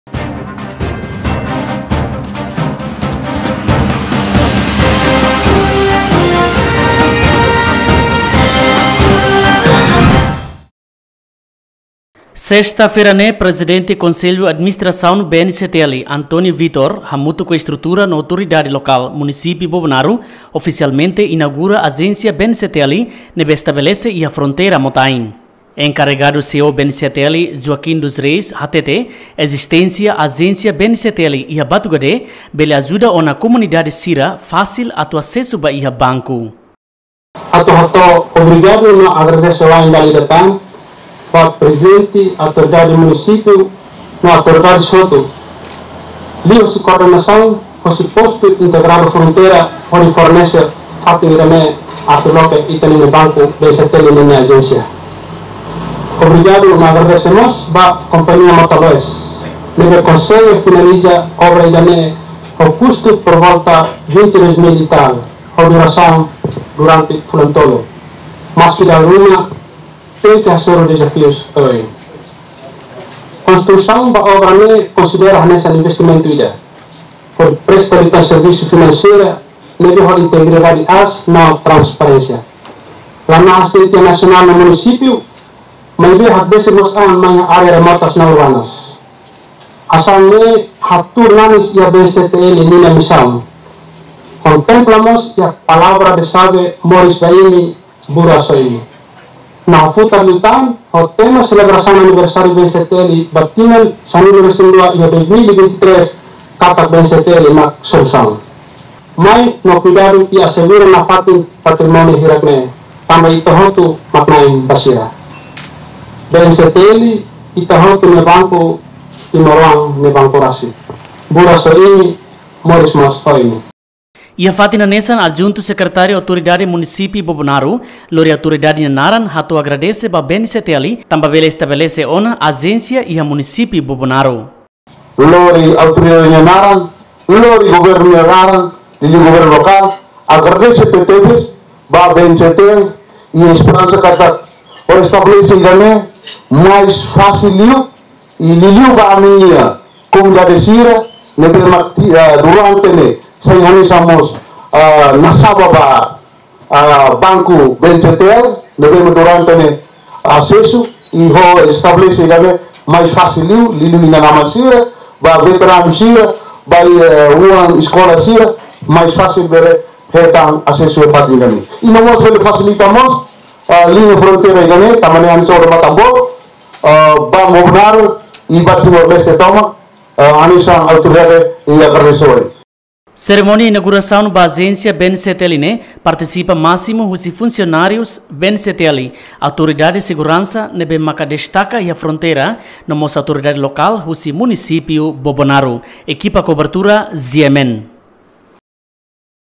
Prezidente BNCTL hamutuk ho AL Munisipiu Bobonaru inagura ajénsia BNCTL iha Batugede
Prezidente Konsellu Administrasaun BNCTL hamutuk ho Autoridade Lokál Munisipiu Bobonaru inagura ajénsia BNCTL iha Batugede